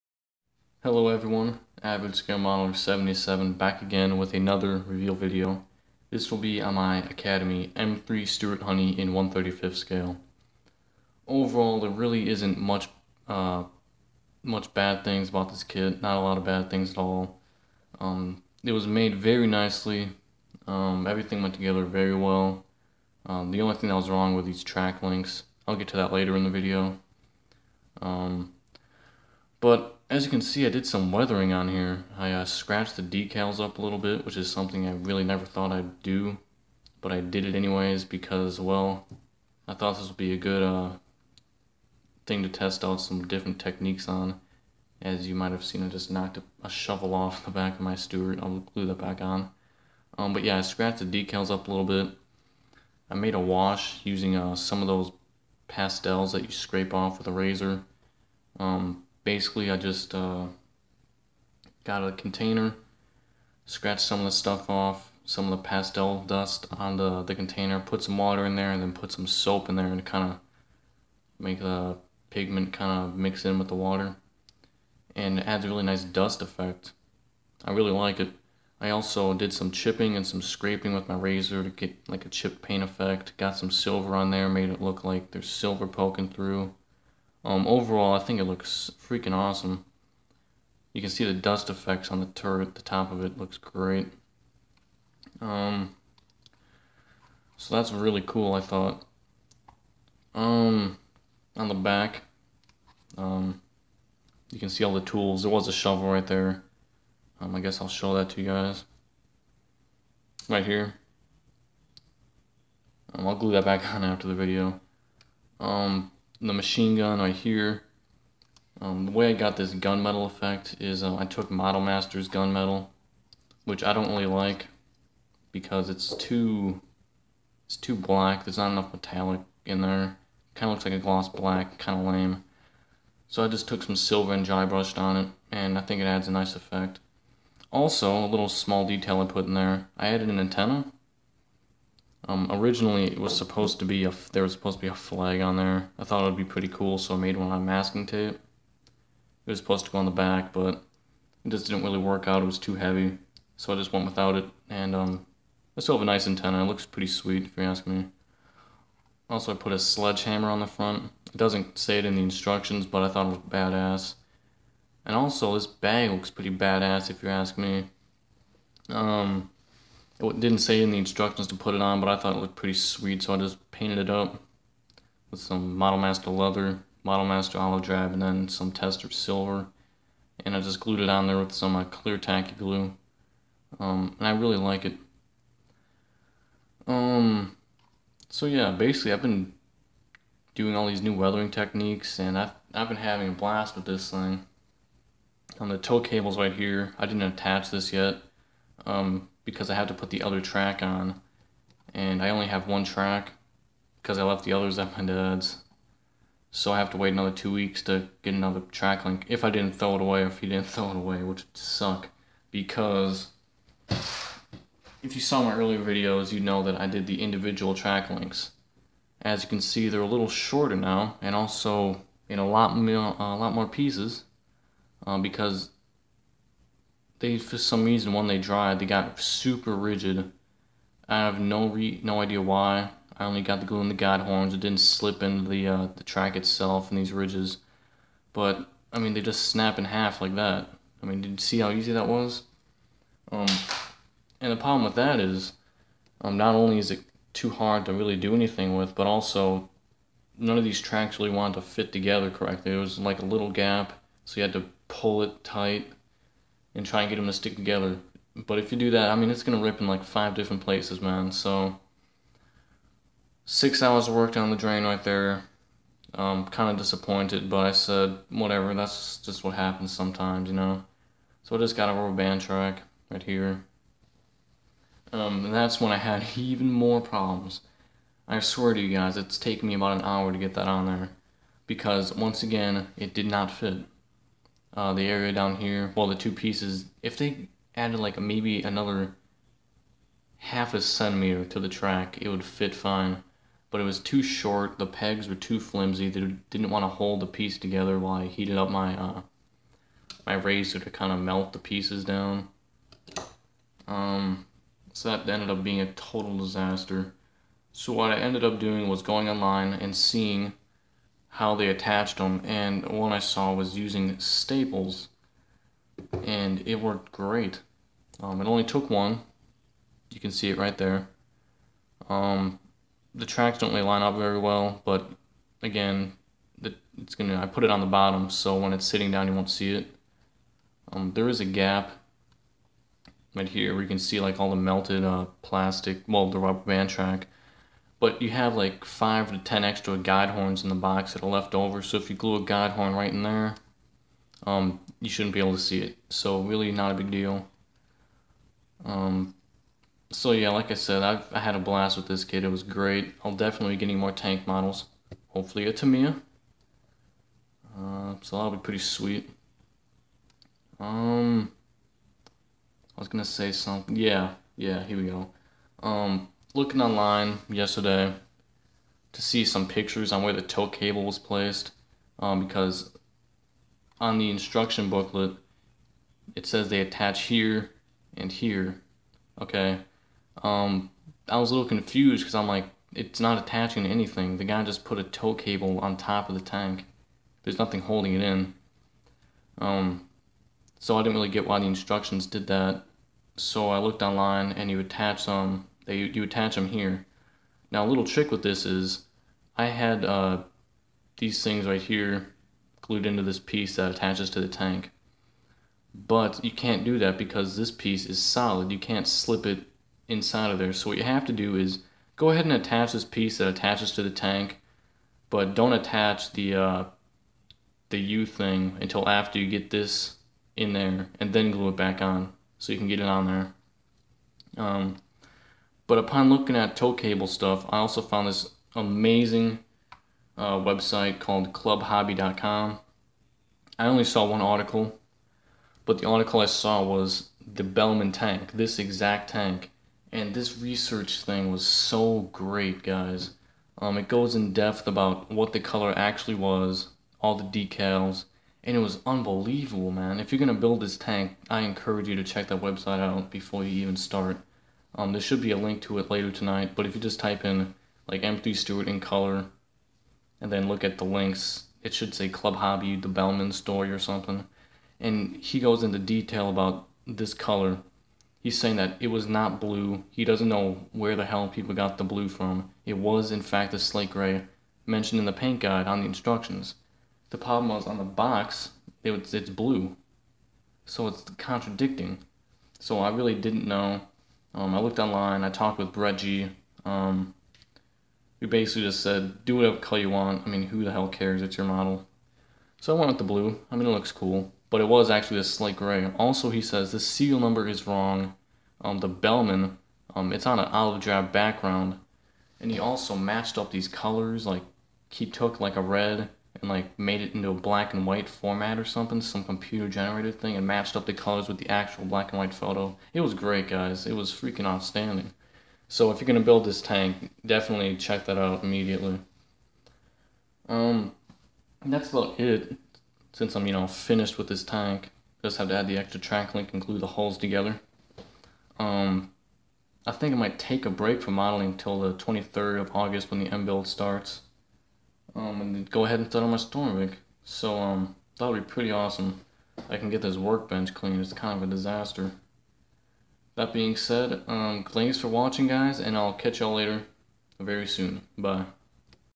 Academy M3 Stuart Reveal
My "finished" Stuart tank. This was a very nice kit with little issues and I highly recommend it.